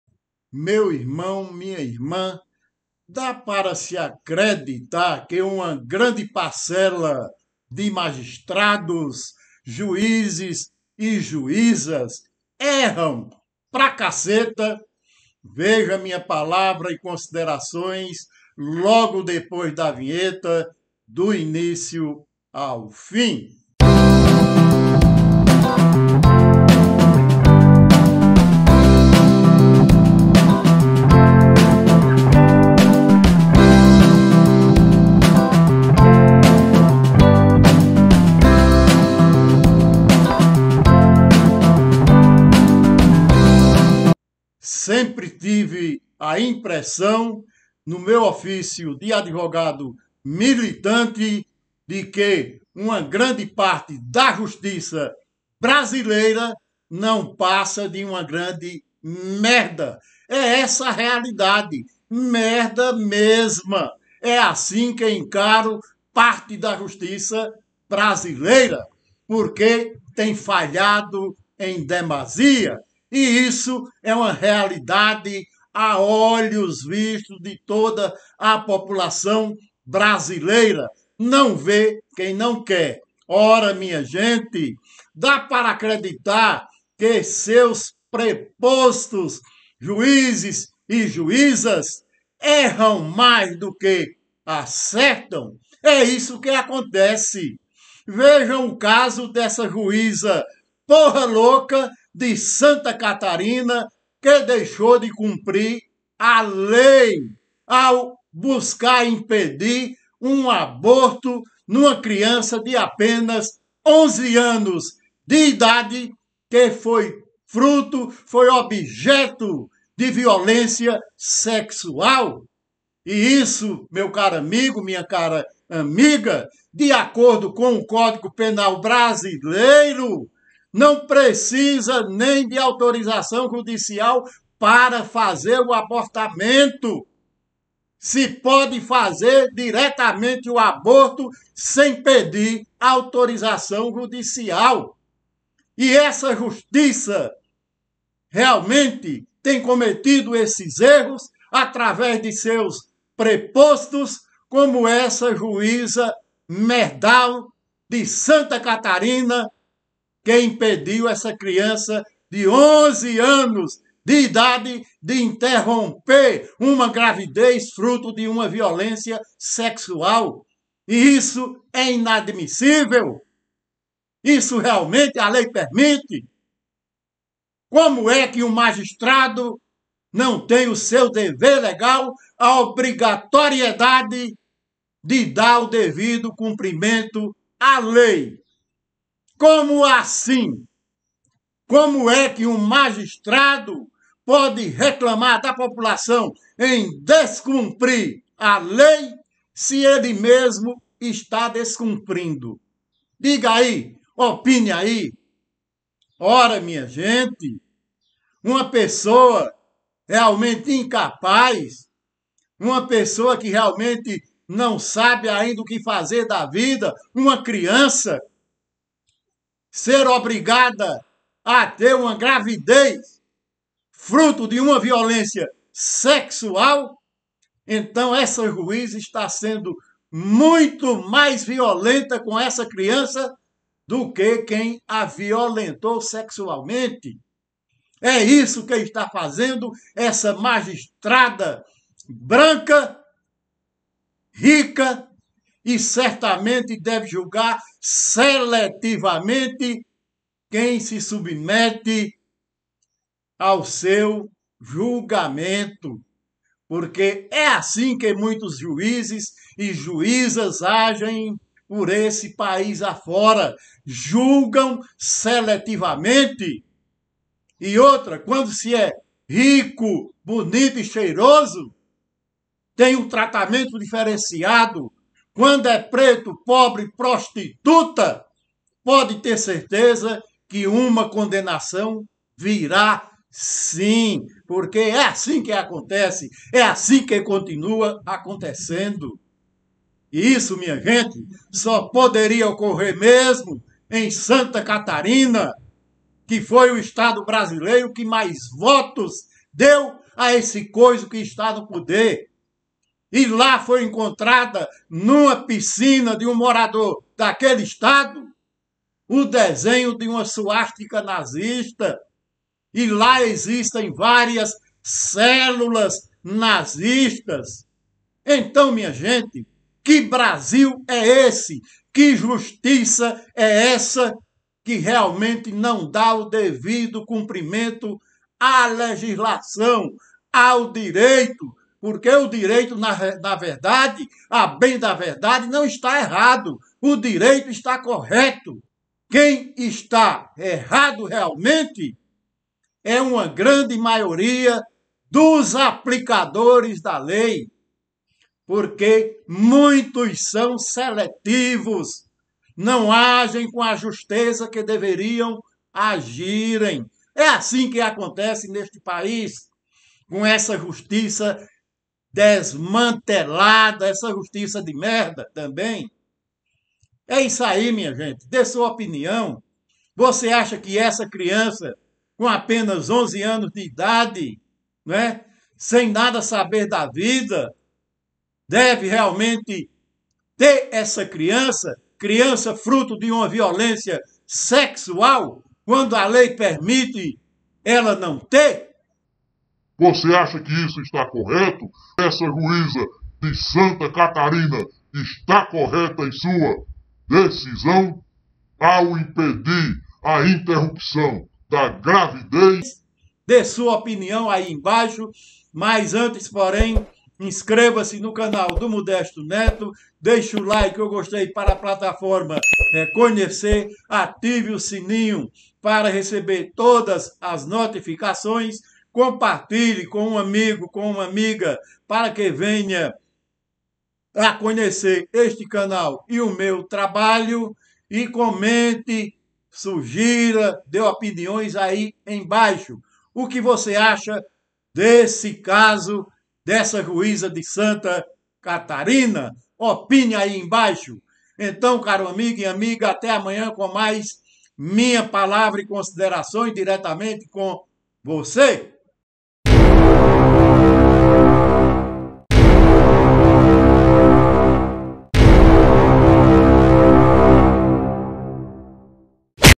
Juíza de Santa Catarina Faz Merda ao Descumprir a Lei! - Canal do Modesto Neto
Um juíza de Santa Catarina, fez merda em seu ofício judicante, ao impedir o abordo de uma criança de apenas 11 anos de idade, sem sequer ter o devido conhecimento de sua existência, que engravidou fruto de violência sexual, exceção em que, o próprio Código Penal, permite o aborto sem sequer a permissividade judicial, mas essa juíza resolvei inovar. Só sendo uma juíza de merda mesmo! Meu muito obrigado!